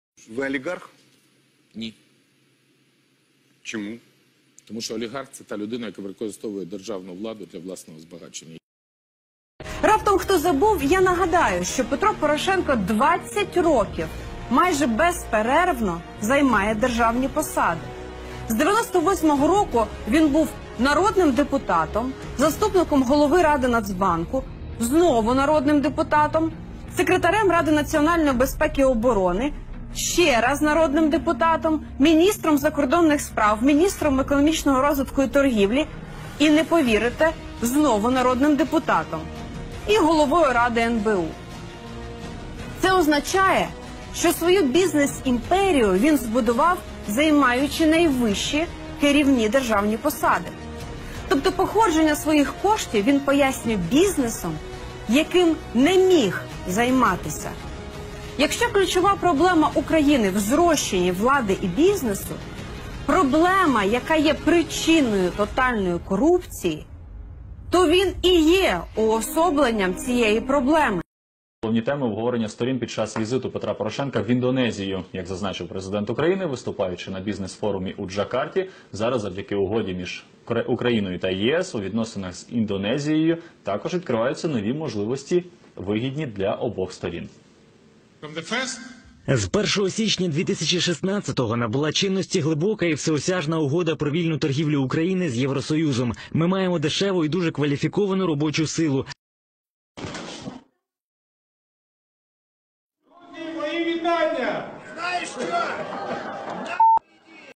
Ви олігарх? -Ні...
Українці не випадково стали найбіднішим народом Європи.
Подивіться це відео до кінця, щоб зрозуміти чому олігархічний тип економіки зацікавлений в тому, щоб населення України було бідним. Українці не випадково стали найбіднішим народом Європи - олігархам потрібна "дешева робоча сила". З цієї і тільки з цієї причини в Україні "європейські зарплати" тільки у чиновників (група підтримки олігархату). Дякуємо за репост.